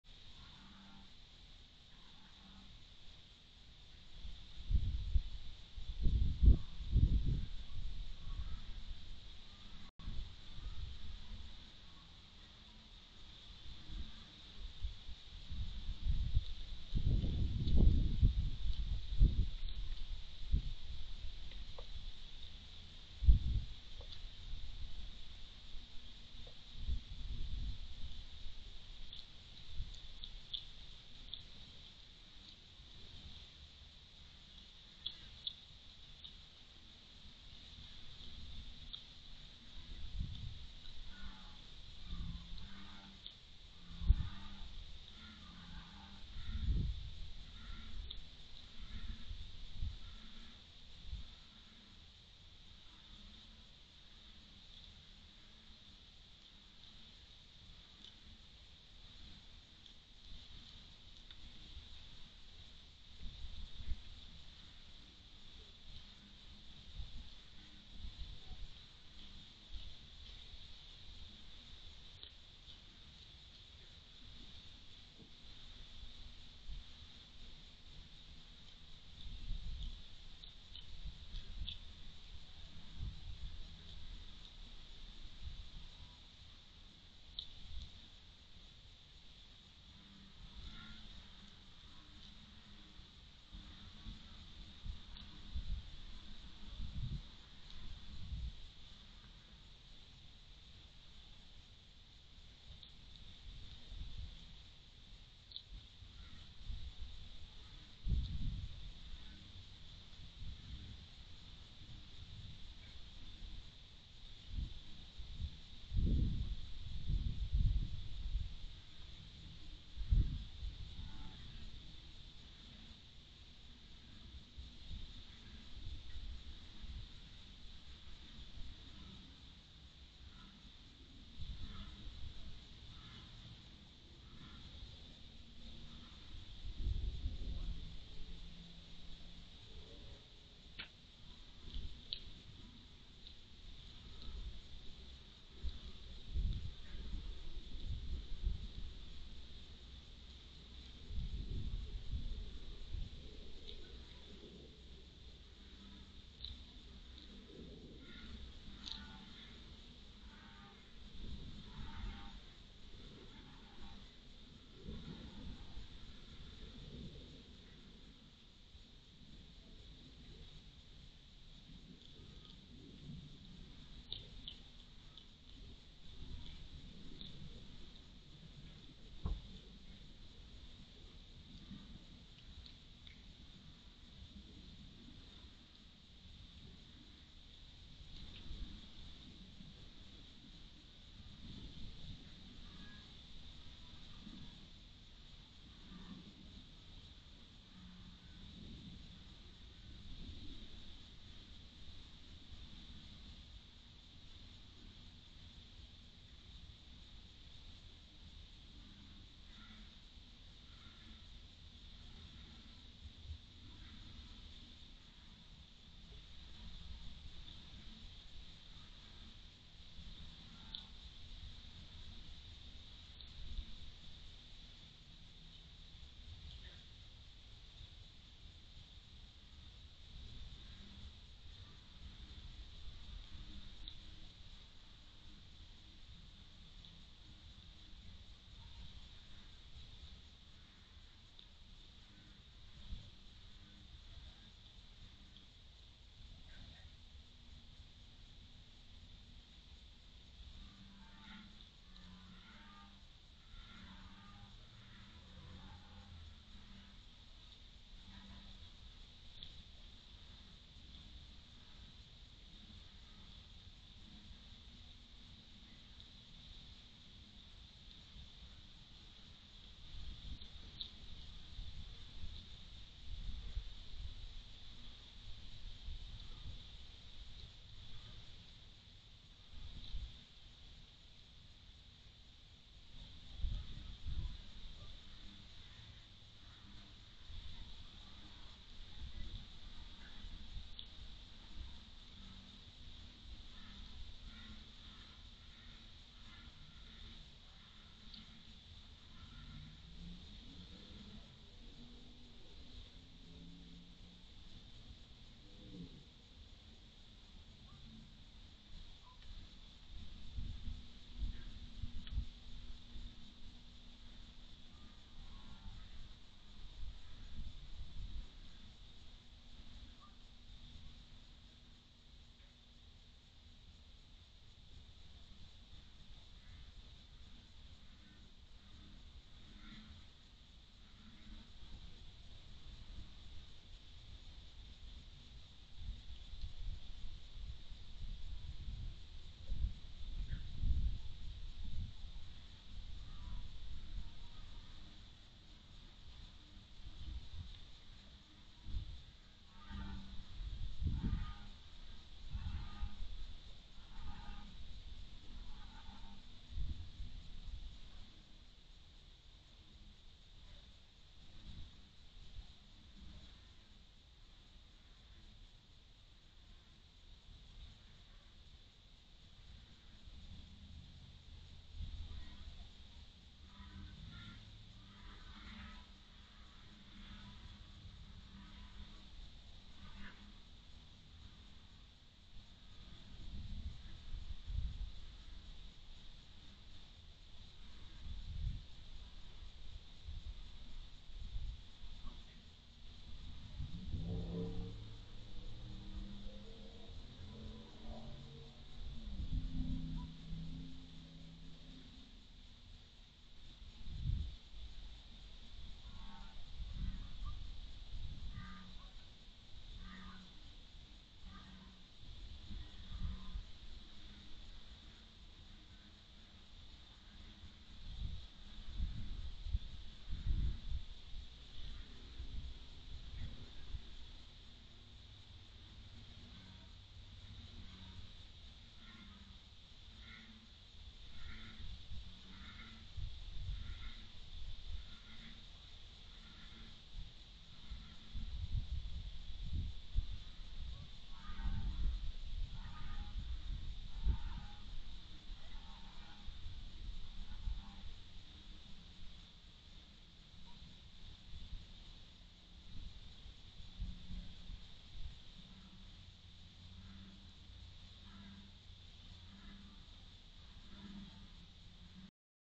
2023 06 11 Mangy fox feeding 3
Mangy fox on Massassauga Road near the Bay of Quinte